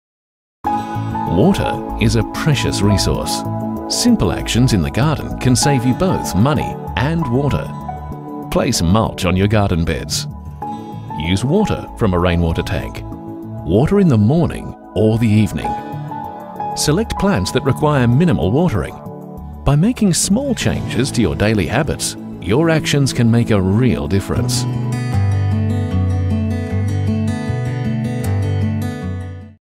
Saving water in the garden
Learn how simple actions in the garden can save both water and money.